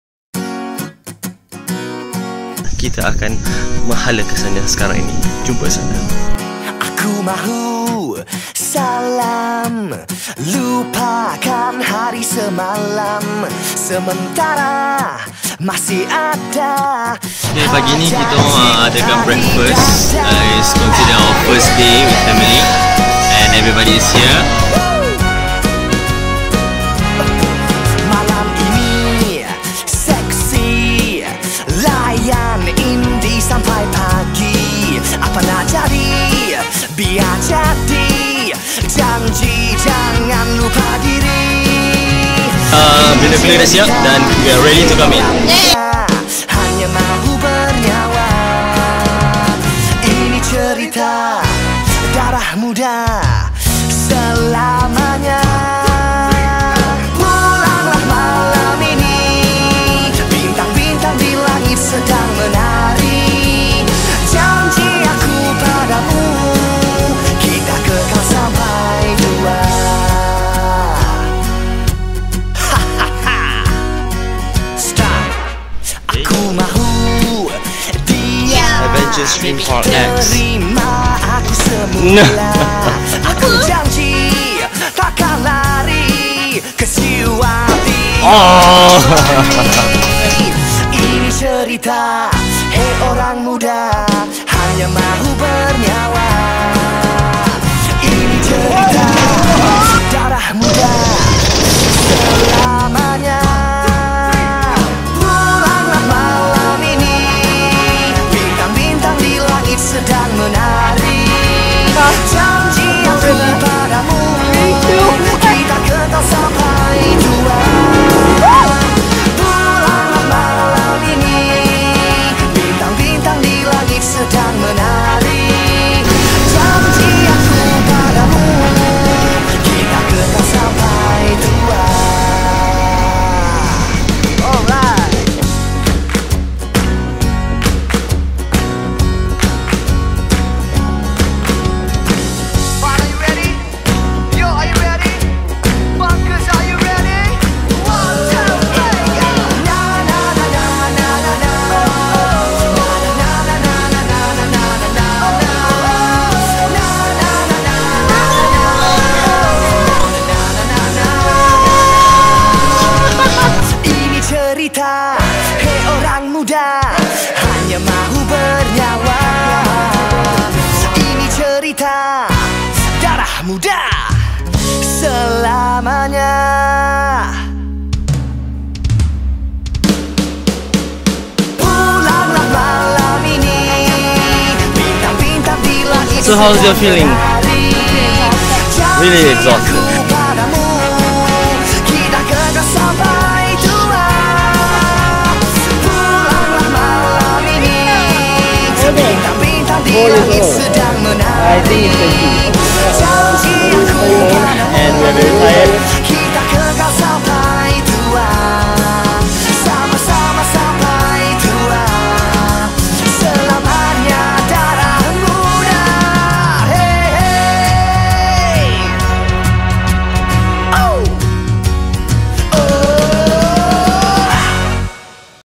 Holiday Getaway with Beloveds' @ Langkawi Island
3 days 2 nights vacation with beloveds in Langkawi Island

Great places makes great memories

List of places found in video:-
1) Dataran Helang
2) Dayang Bay Serviced Apartment & Resort
3) Langkawi Sky Cab
3) Langkawi Adventure and X-treme Park
4) Langkawi Sky Bridge
5) Makam Mahsuri

Hope to enjoy the video